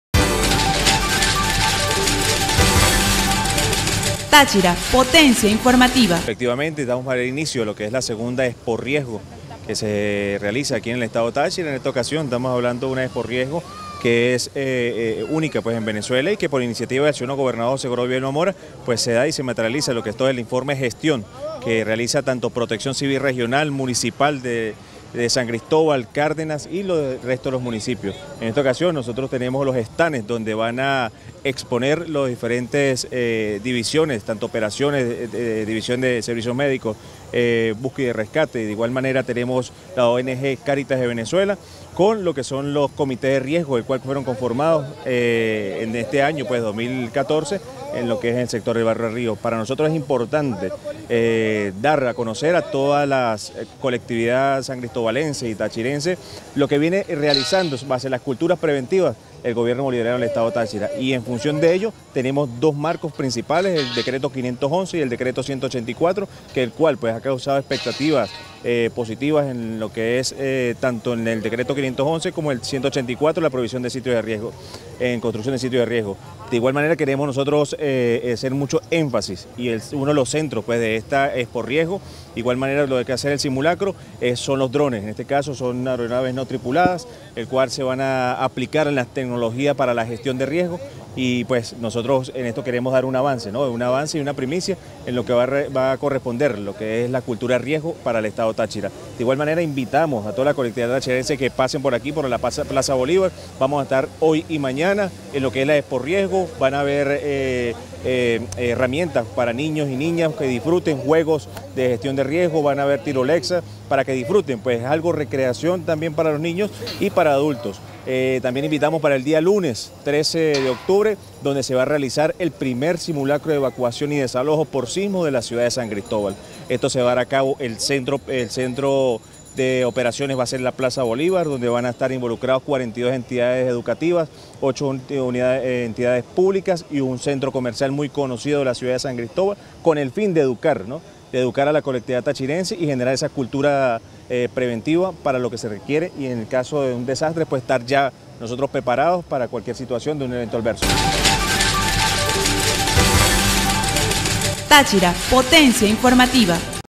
Fomentan cultura preventiva en II Expo Riesgo 2014
Con todo éxito se desarrolló en la Plaza Bolívar de San Cristóbal la segunda Expo Riesgo organizada por el Instituto Autónomo Protección Civil del Estado Táchira con el fin de fomentar una cultura preventiva ante cualquier situación de contingencia. Más información